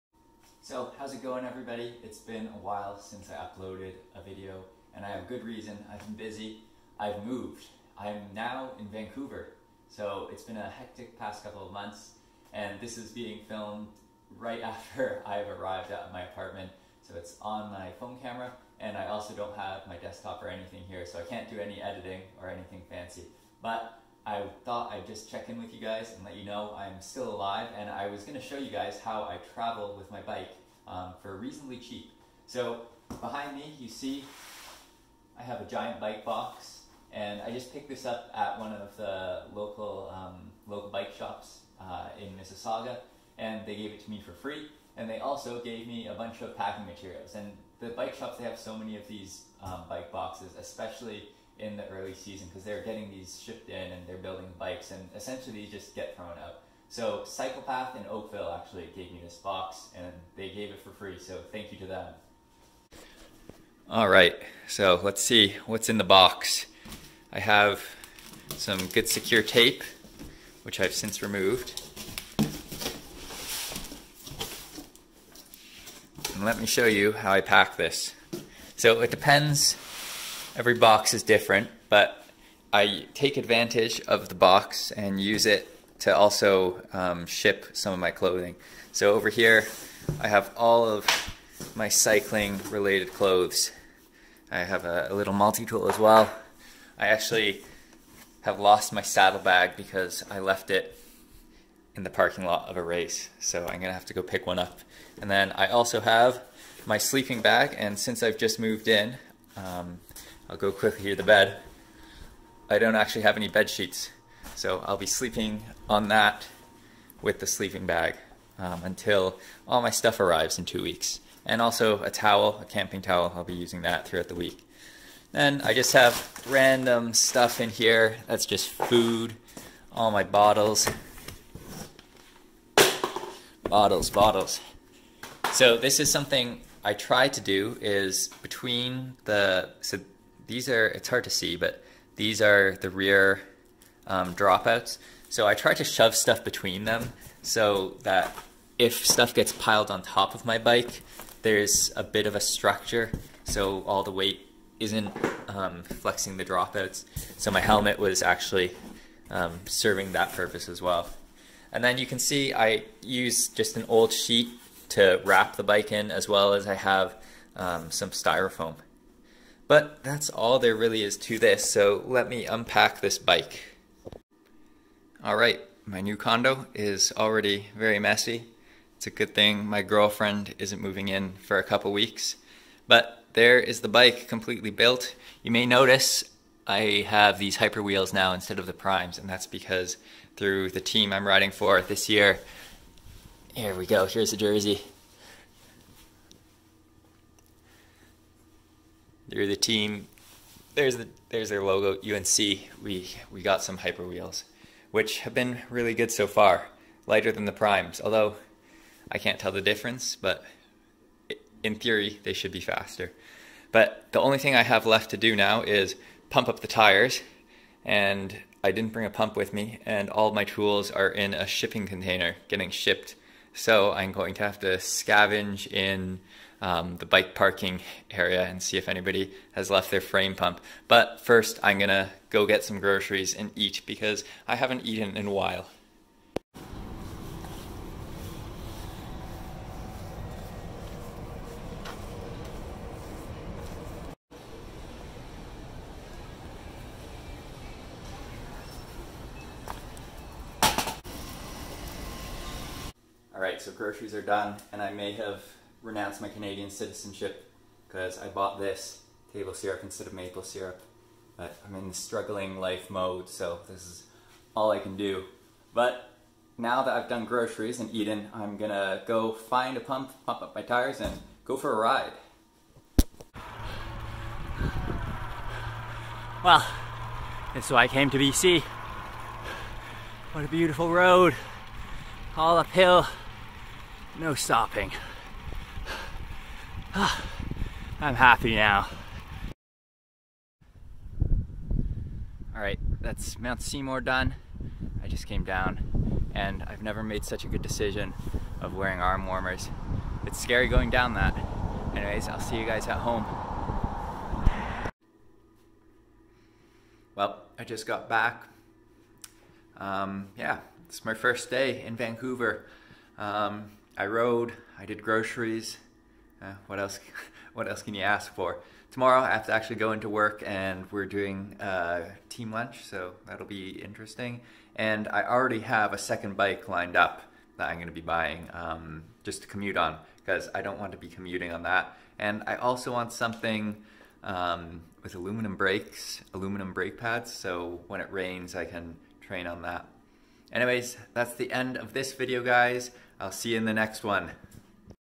Flying my bike to the other side of the country
I've moved across the country to Vancouver! This is how I fly with my bike on the cheap, and also help the environment out!